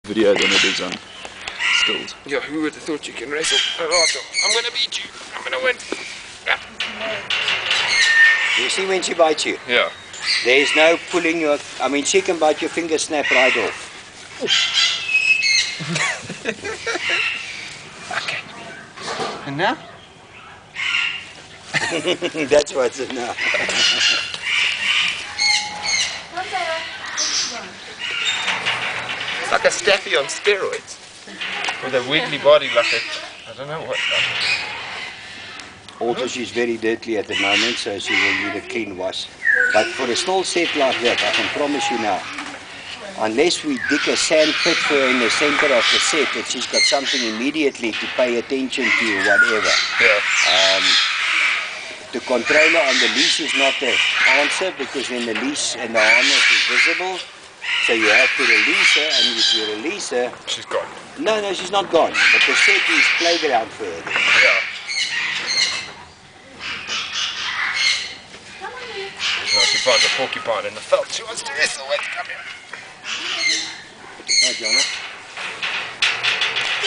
Honey Badger Wrangling....
Touted as the most viscious and destructive, and toughest creatures on earth, the honey badger is feared by pretty much everything, and scared of nothing, quite happily taking on anything in its path. this little sucker named Beatrix, is probably the only workable Honey Badger in Africa. I've been obsessed with them since I was a kid, so getting the chance to actually handle one, was a rush of note... I cannot even begin to describe how tough, solid, and squirmy she was.